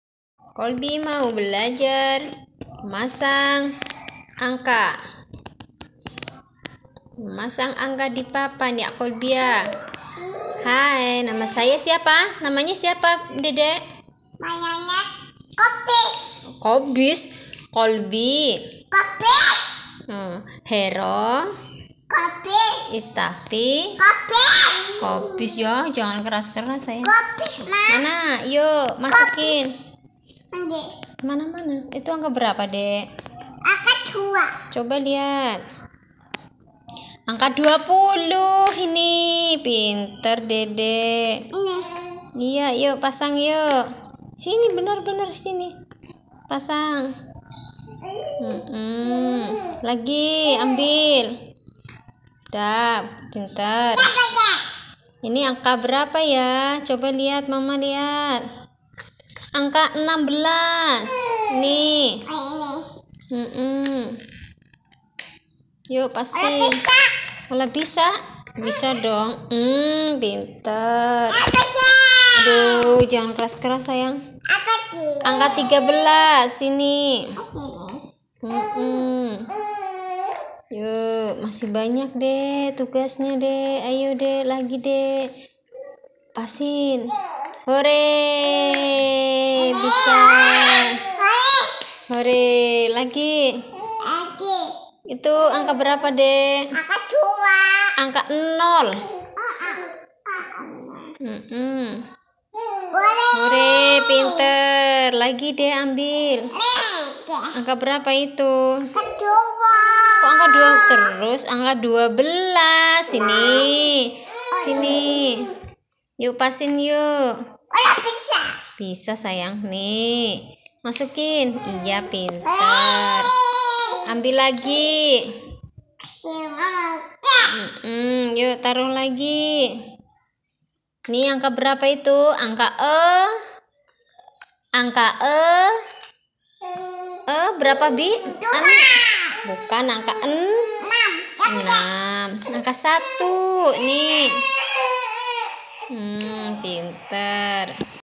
qolbi belajar menghitung
menghitung angka 1-10 ok selaamat belajar teman2.